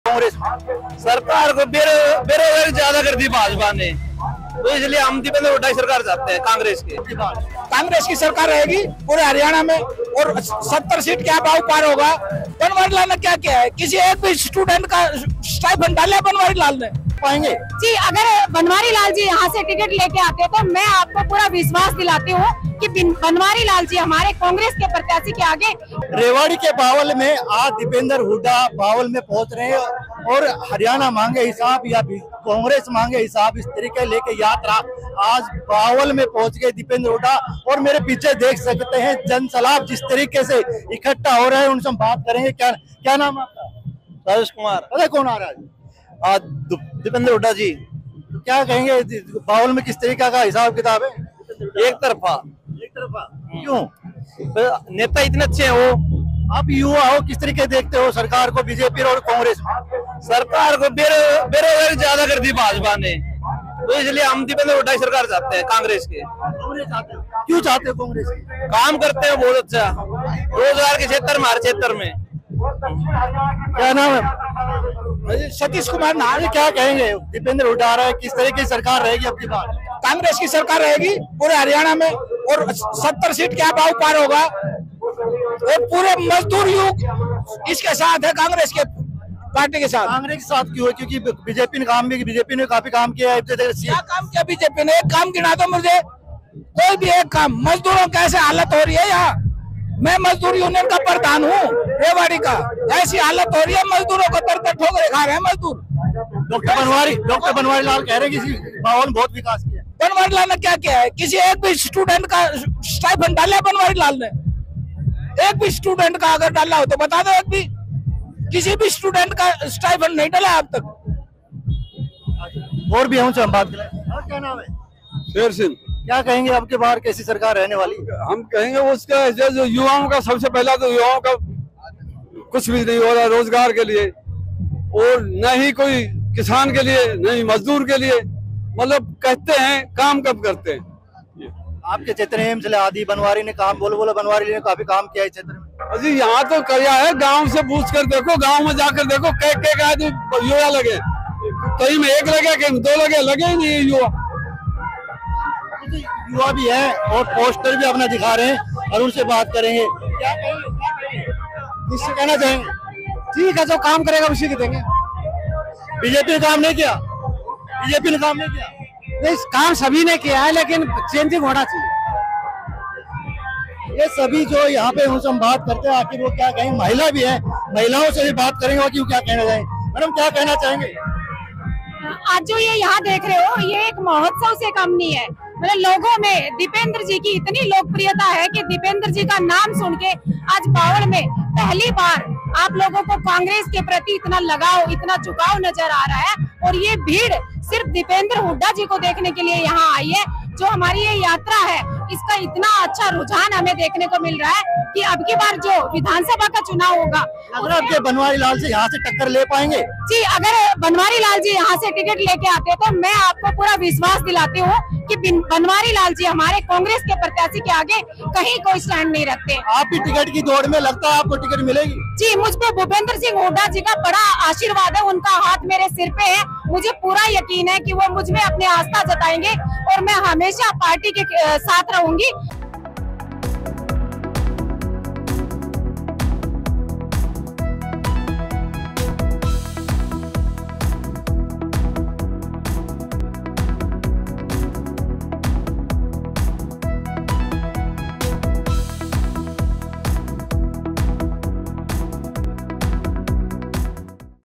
Rewari जिले में पहुंची Deepender की यात्रा, सुनिए क्या बोले अहीरवाल के लोग?। Haryana Tak
दीपेंद्र हुड्डा अपनी हरियाणा मांगे हिसाब यात्रा लेकर बीते रोज बावल पहुंचे थे। अहीरवाल में उनकी यात्रा का ये पहला पड़ाव था, दीपेंद्र ने यहां भाजपा सरकार को अग्निवीर और रोजगार के मुद्दे पर जमकर घेरा। उनकी यात्रा में शामिल हुए लोगों में दिखा कांग्रेस के जोरदार उत्साह।